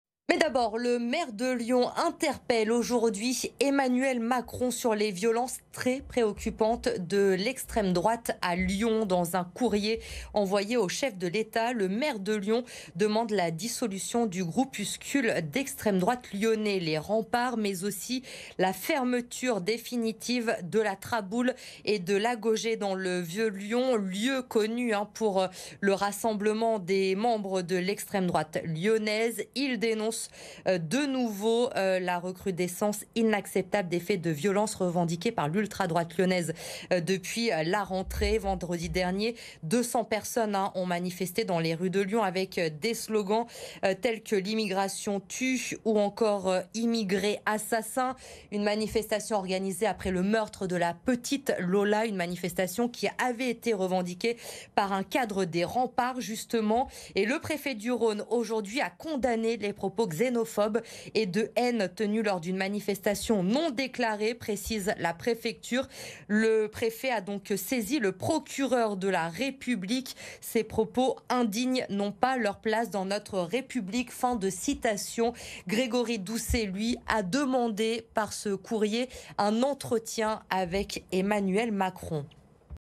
Le maire de Lyon interpelle Macron
Dans un courrier adressé au président Emmanuel Macron, Grégory Doucet, maire de Lyon, demande des mesures contre les groupes d'extrême droite lyonnais